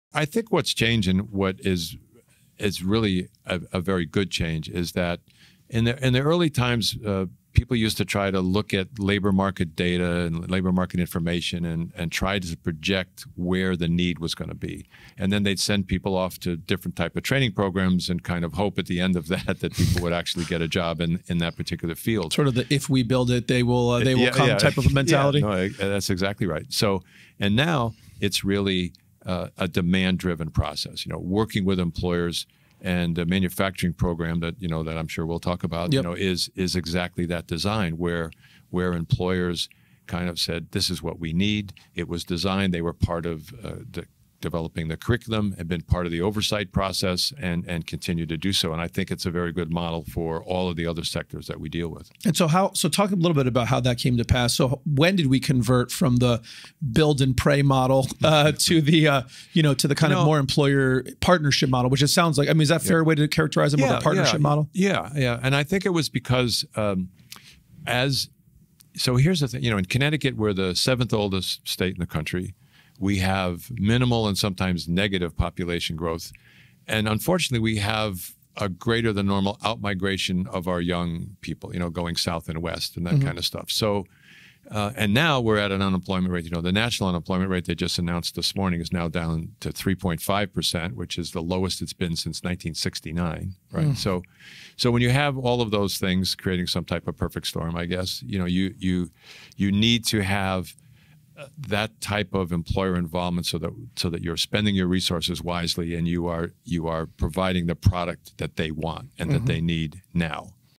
What is Changing in Workforce Training in CT?
Things are changing in workforce development in CT. Improvements are being made quickly to match the needs of employers. There's so much good stuff going on!

Find out more from Bill Villano, President of Workforce Alliance, on this episode of the Made in America podcast.